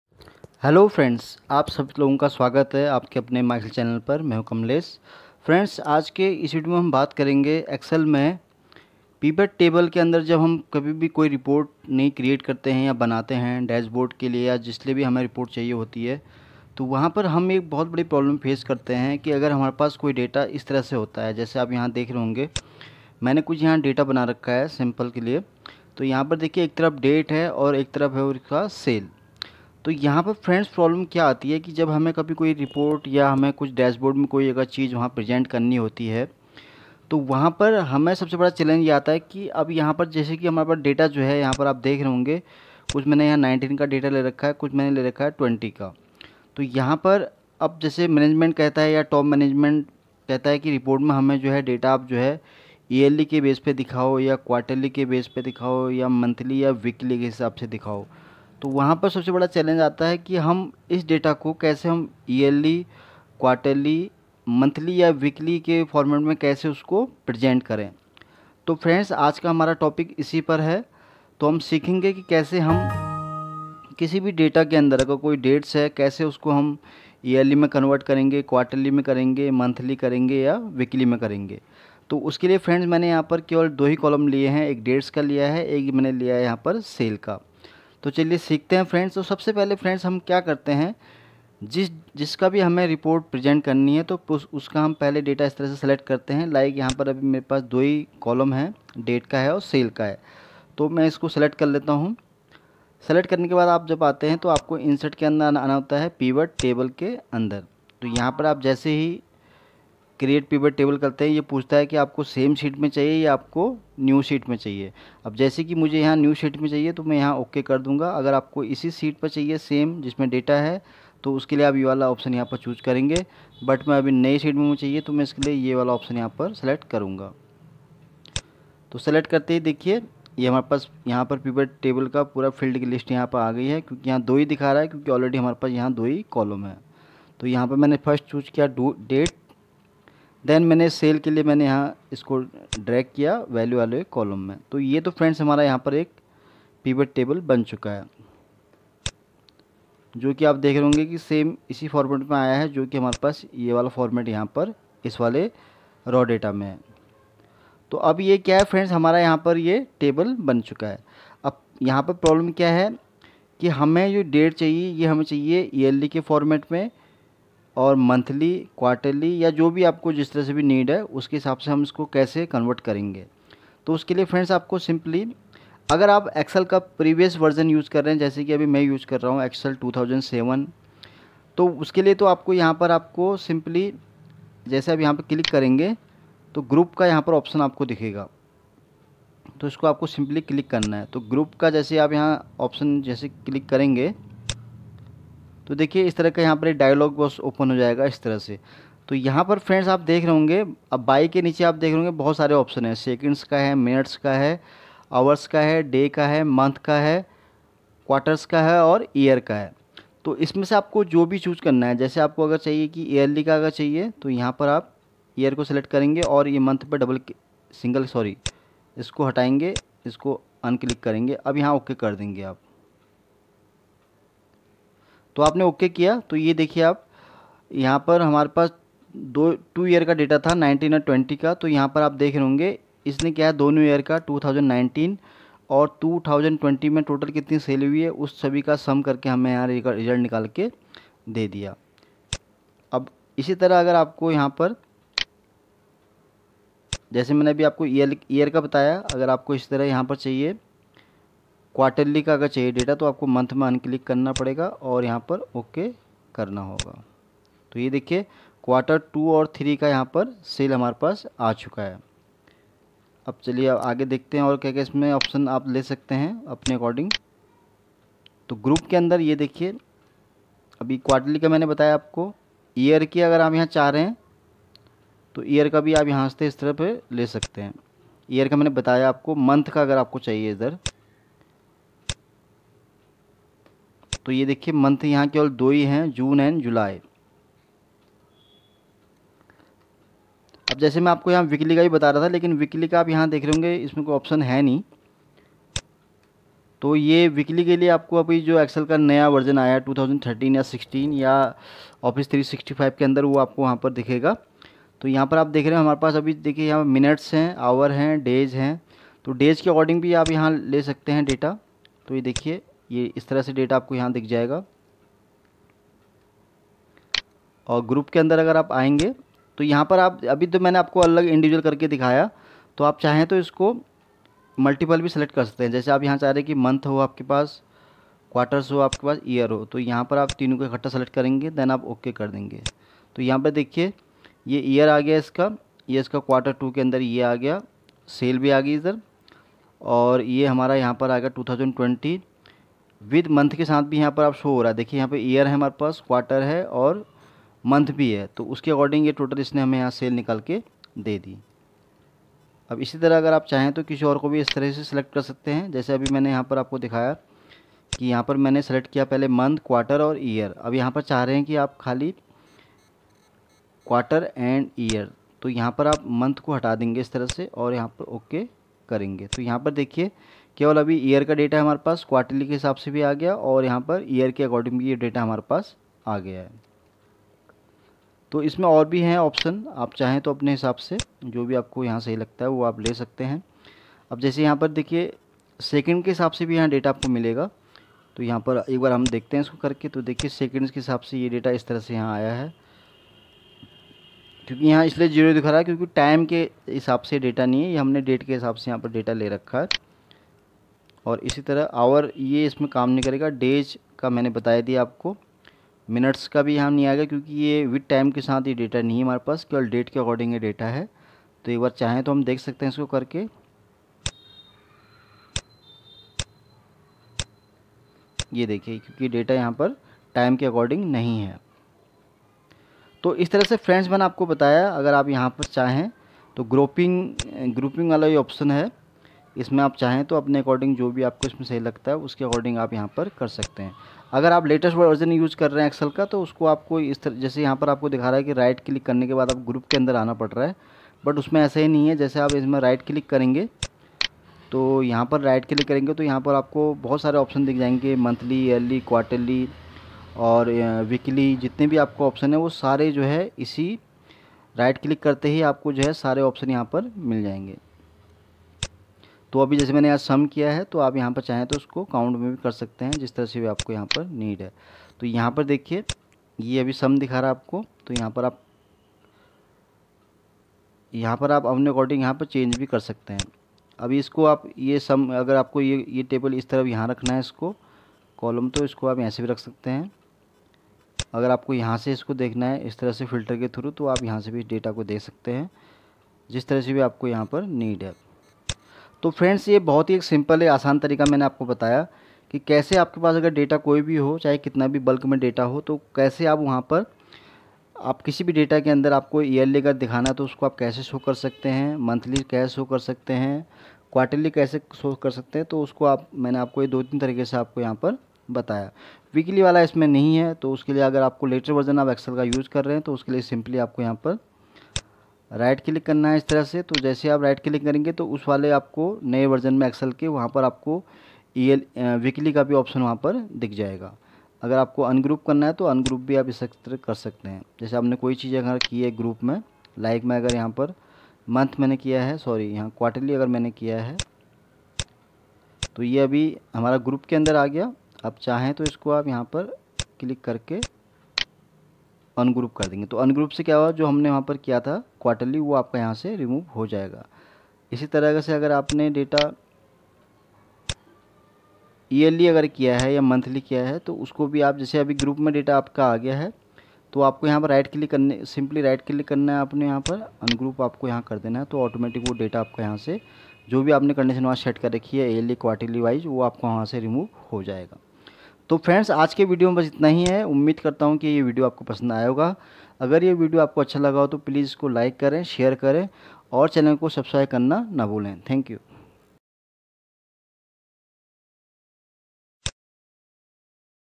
How to Group Dates in Pivot Tables by Yearly, Quarterly and Monthly#pivot
आज के इस video में आप Pivot Tables में Dates को Yearly, Quarterly, Monthly में बदलना सीखेंगे!

Please visit our YouTube Channel for more Excel videos.